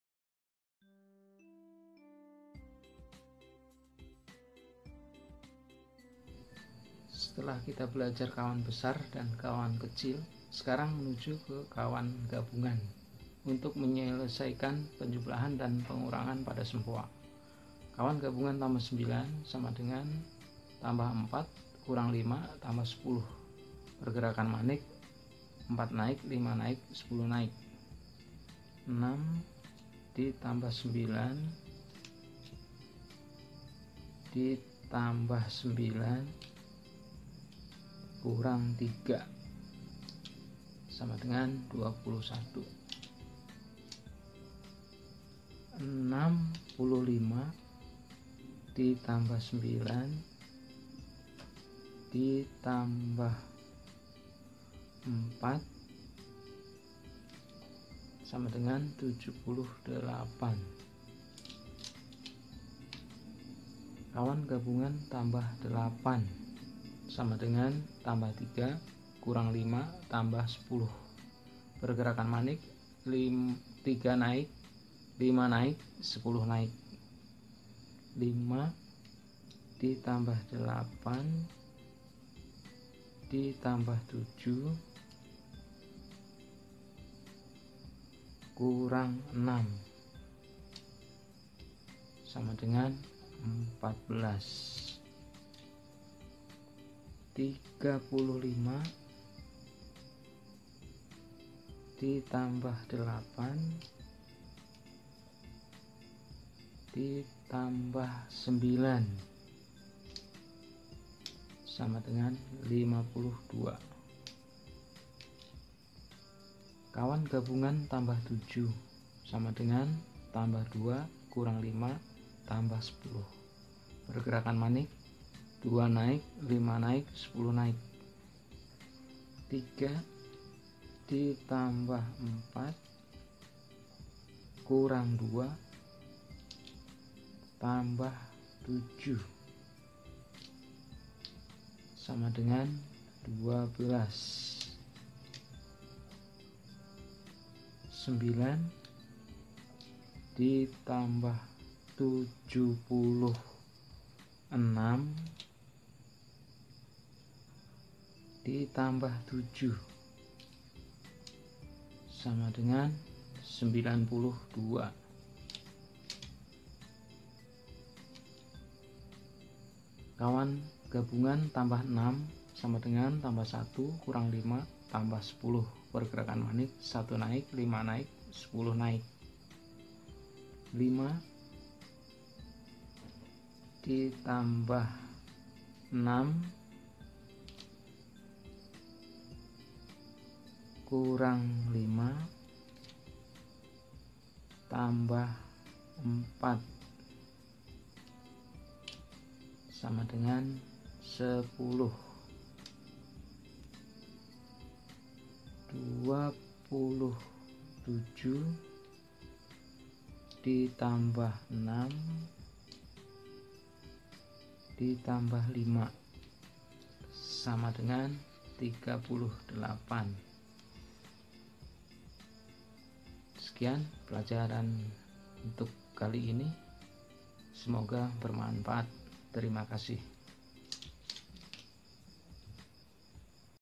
Belajar Berhitung Dengan Sempoa Tahap 3 ( Lanjutan 4, Kawan Gabungan )
Tutorial Sempoa
Belajar Berhitung Dengan Bantuan Kawan Gabungan +6, +7, +8 & +9